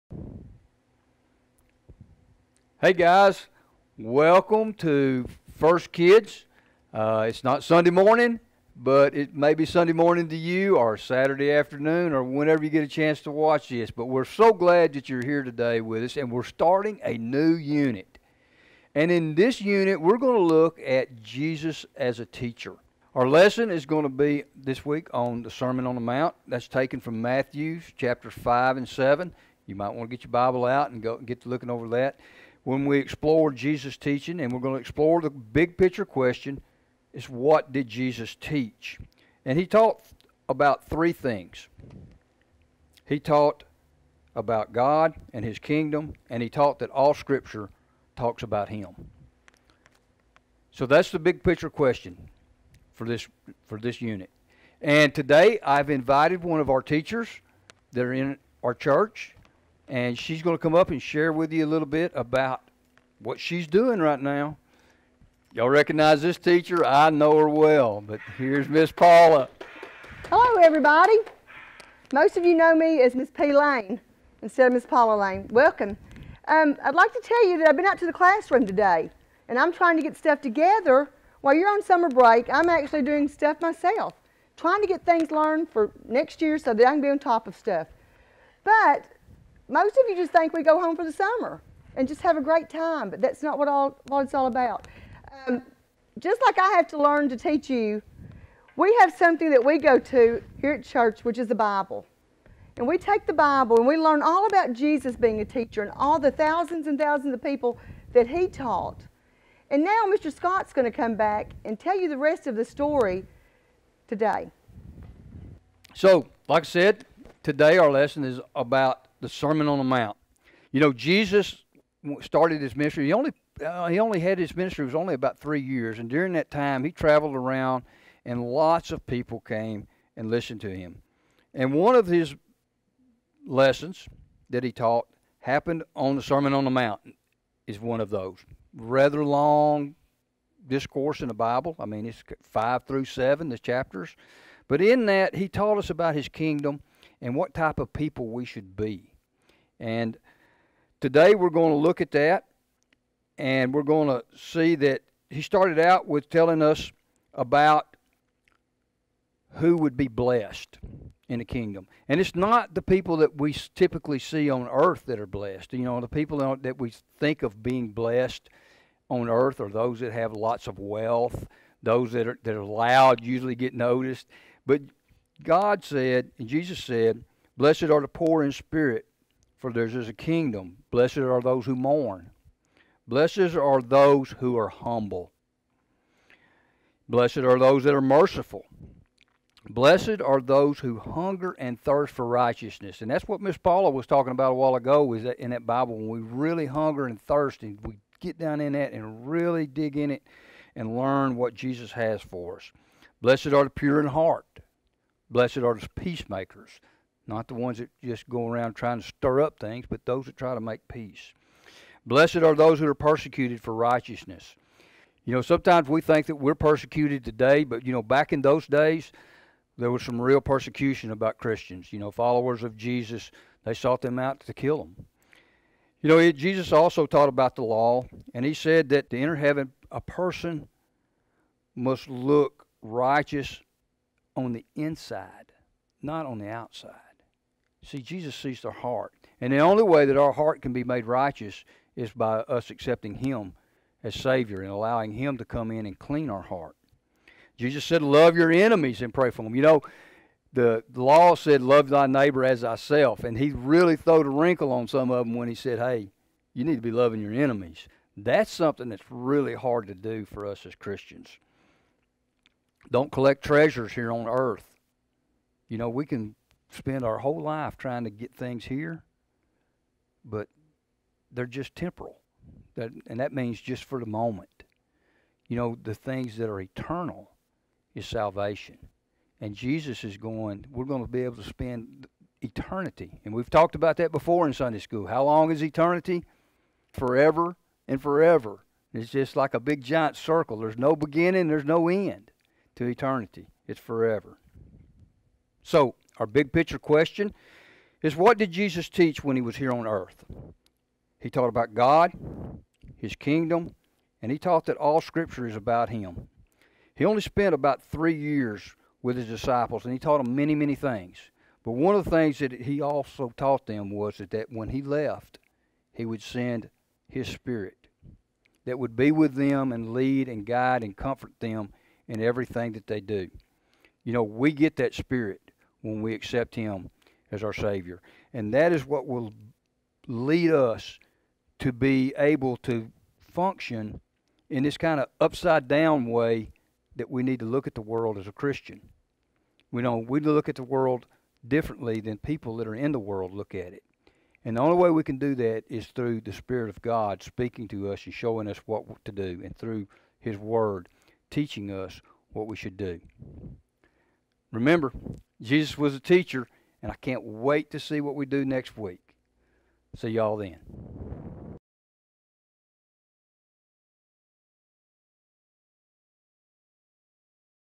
First Kids Life Groups - Sermon on the Mount
Join Mr. Scott and special guest Mrs. Paula this week for a lesson on the Sermon on the Mount.